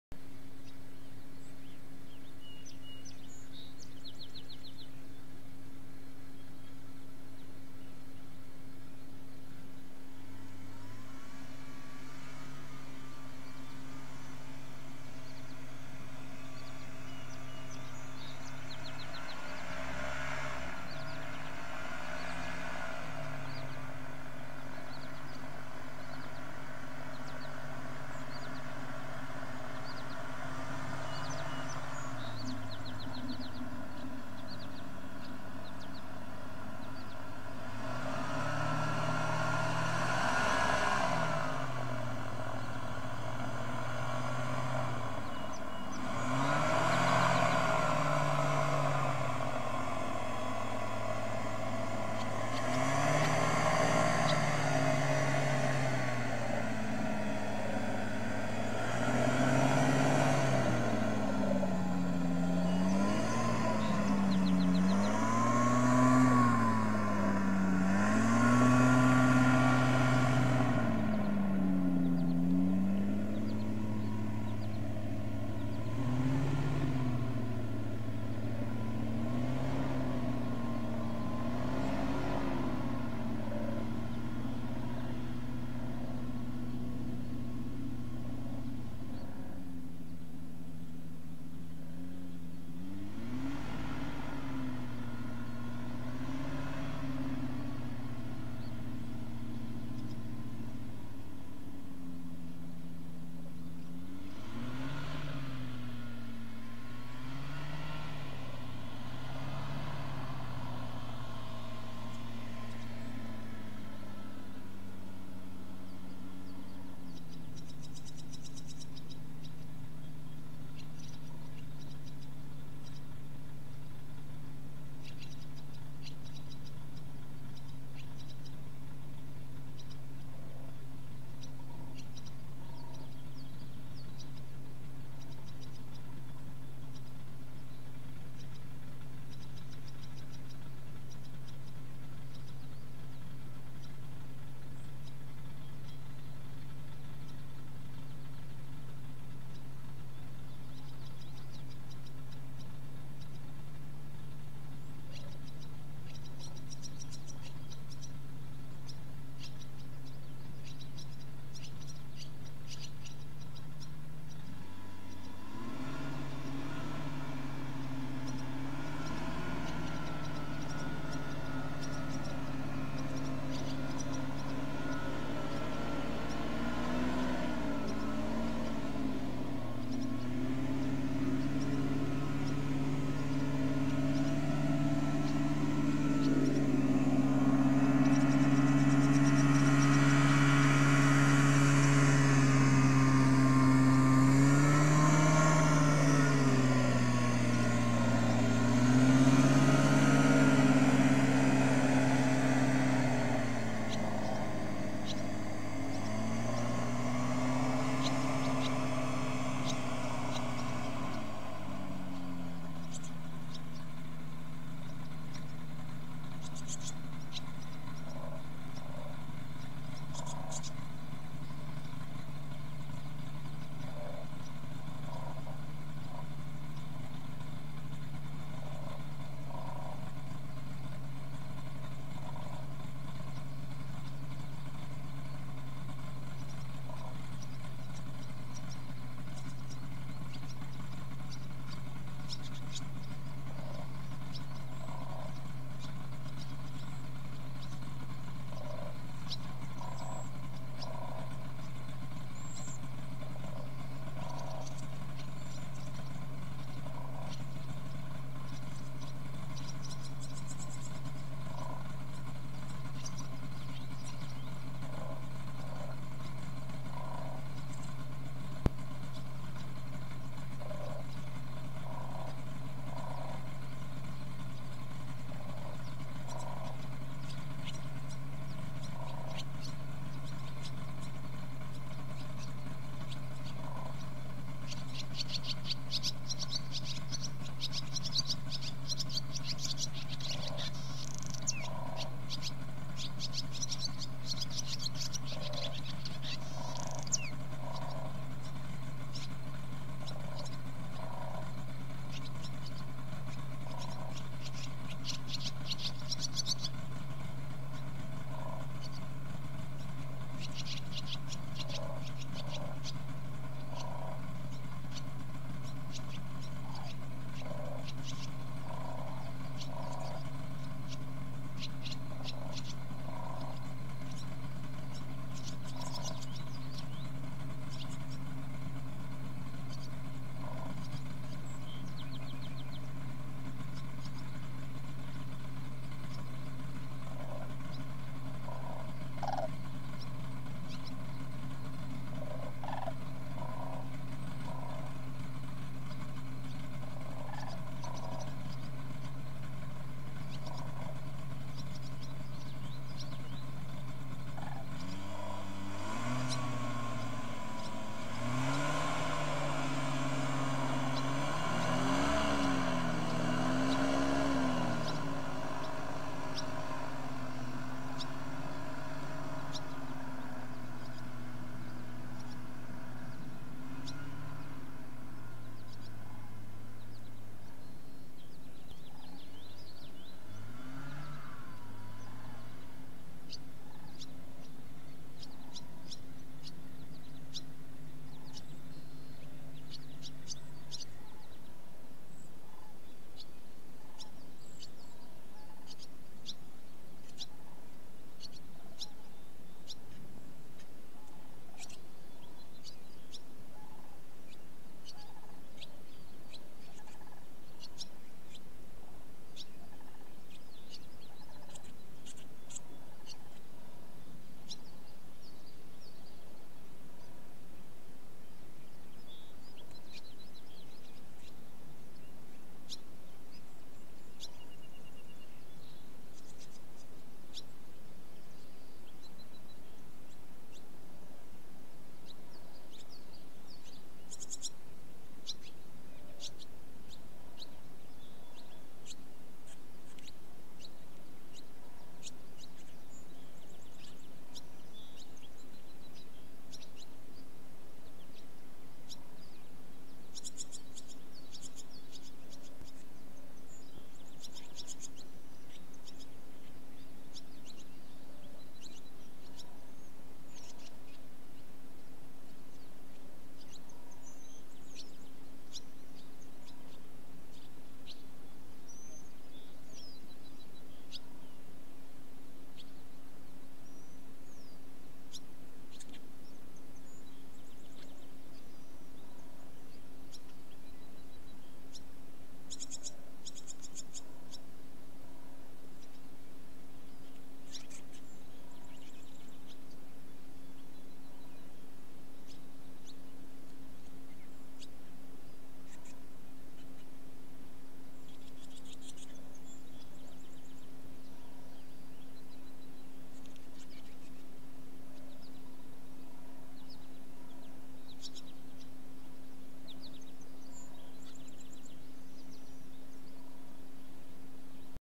070812 OMPenCam 0708
This camera ( ‪ ) records the awe inspiring story of a dedicated team of conservationists at Operation Migration ( ‪ ) and their mission to save the highly endangered Whooping Crane. You'll be able to follow the training of the cranes leading up to their first migration south. A journey that which will be broadcast LIVE and in-flight, you'll be up in the air with the cranes!